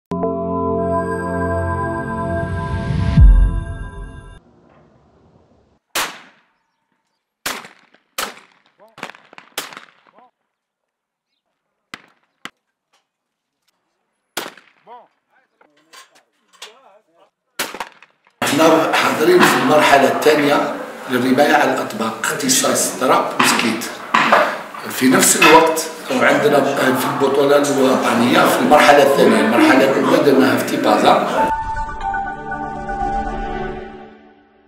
باتنة: انطلاق منافسات المرحلة الثانية للرماية على الاطباق بمشاركة اكثر من 100 رامي
باتنة (وأج)- انطلقت بميدان الرماية ببلدية جرمة بولاية باتنة منافسات المرحلة الوطنية الثانية للرماية على
الأطباق ''خندق أولمبي'' و''سكيت أولمبي'' بمشاركة أزيد من 100 رامي يمثلون 14 رابطة للرماية الرياضية من عدة ولايات من الوطن .